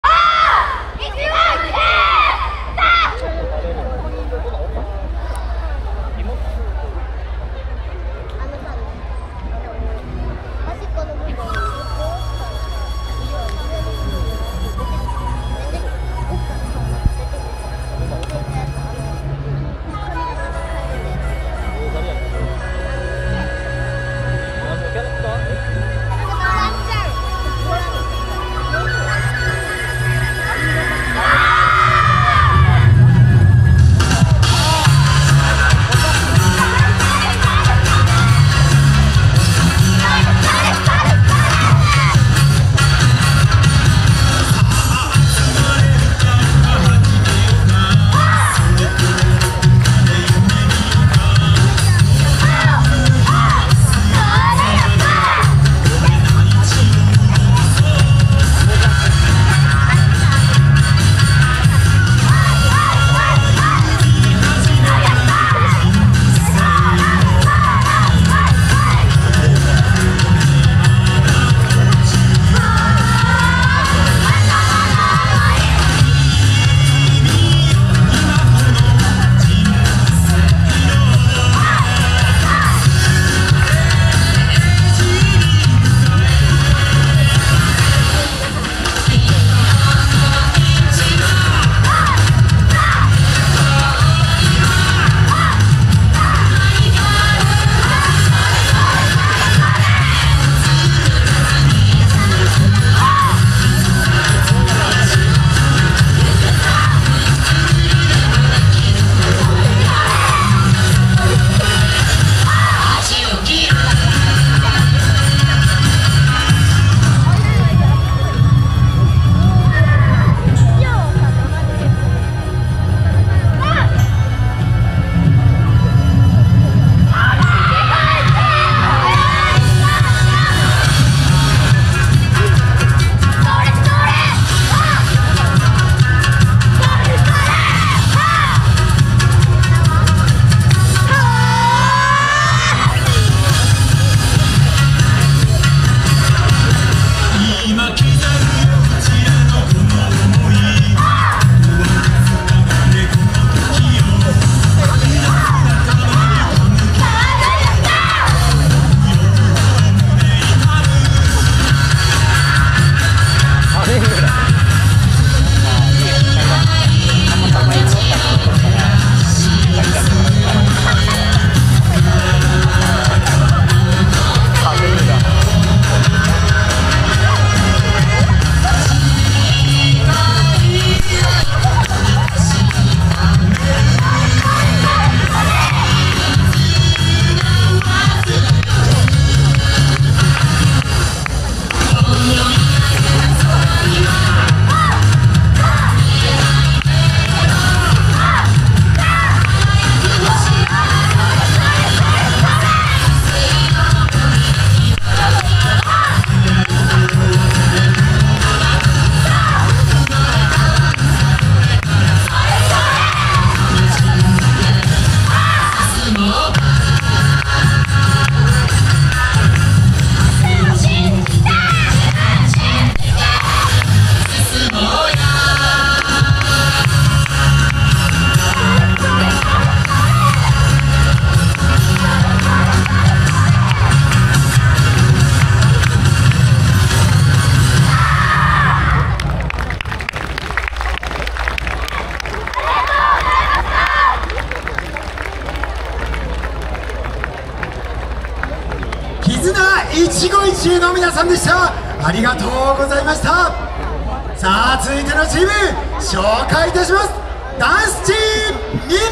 30 絆〜一期一縁〜 四日市よさこい祭り2024［４Ｋ］ wide
2024/11/17 第２０回四日市よさこい祭り